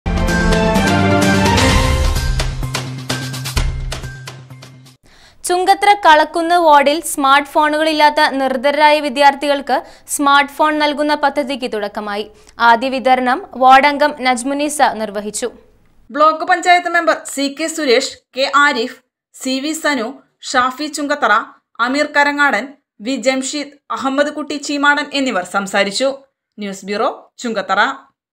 ചുങ്കത്തറ നിര്‍ദ്ധനരായ വിദ്യാര്‍ത്ഥികള്‍ക്ക് സ്മാര്‍ട്ട് ഫോണ്‍ നല്‍കുന്ന പദ്ധതിക്ക്തുടക്കമായി.
ONETV NEWS